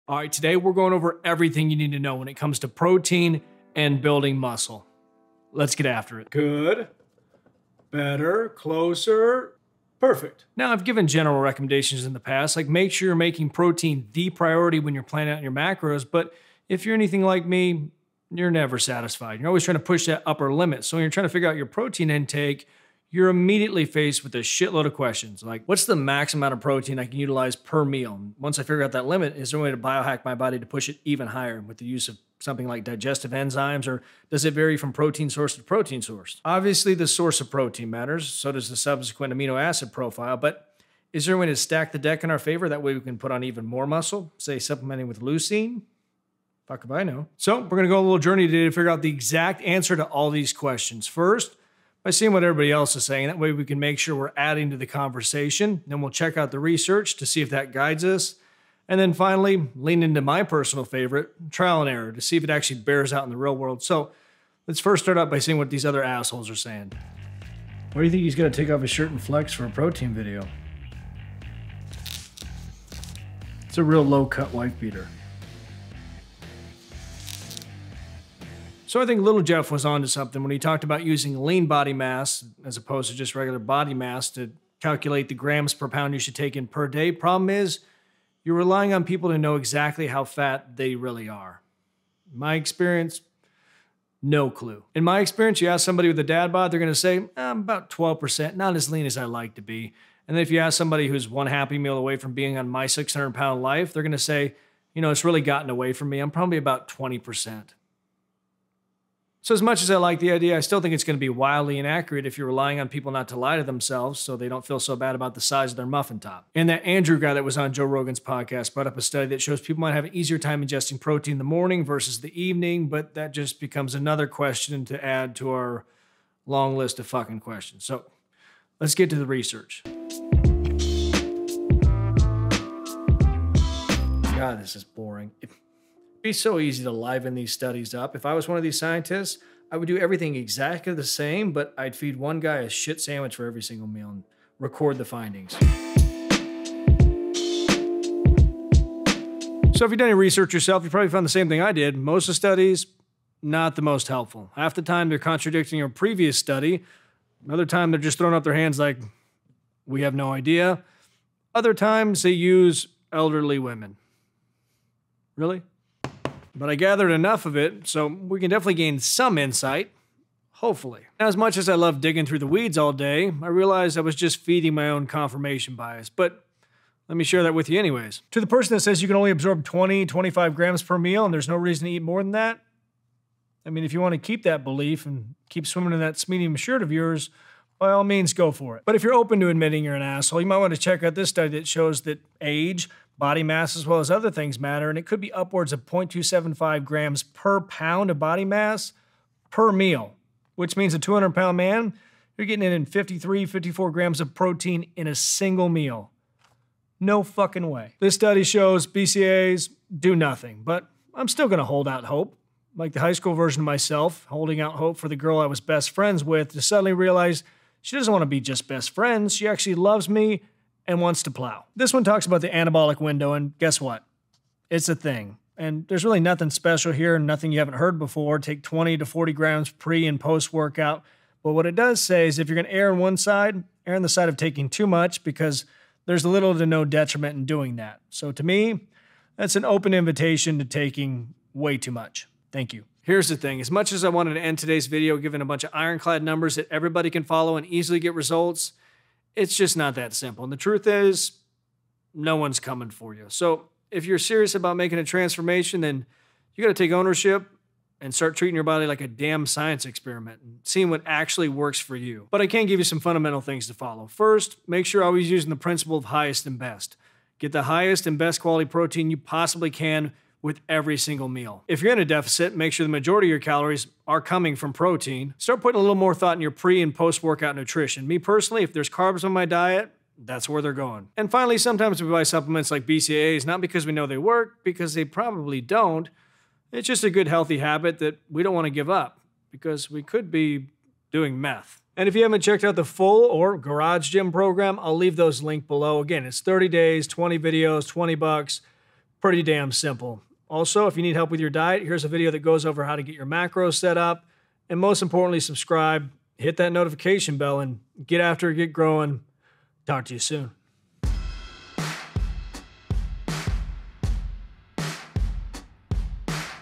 Everything You NEED To Know About Protein & Building Muscle!
Alright, today we're going in-depth on everything to do with protein and building muscle so let's get after it.

I dug through quite a few case studies and here are a few of the takeaways. The amount of protein you can utilize per meal depends on a lot of factors and it isn't as simple as saying "you can only absorb 25-30grams" because that's not true...

Also, BCAA's might not do much in regards to protein synthesis but I still find myself going back to them.

And finally, your pre and post-workout nutrition "the anabolic window" really matters so start building your meal plan around these meals.

If you need help with your diet and need to lose fat, watch this video!
SIMPLEST Way To LOSE FAT (EAT LIKE THIS!)